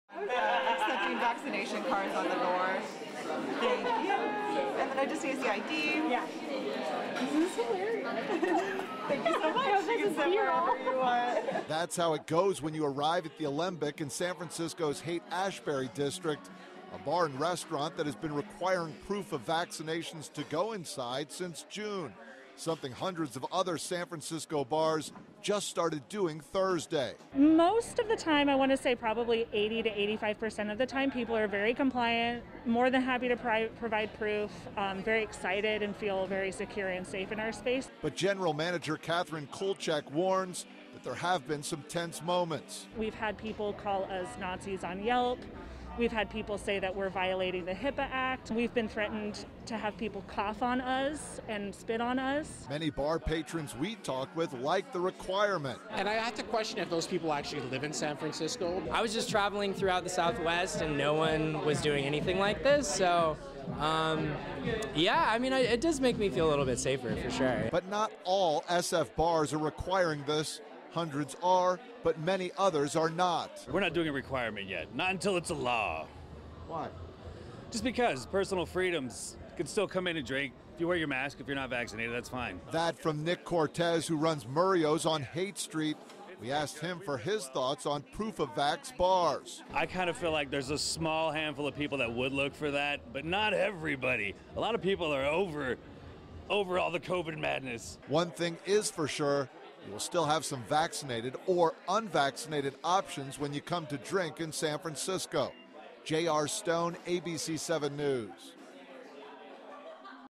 100s Of SF Bars Now Requiring Proof Of Vaccination Or COVID-19 Test 7-29-21
Hundreds of San Francisco bars are now requiring proof of COVID-19 vaccination or a COVID-19 test if you would like to go inside.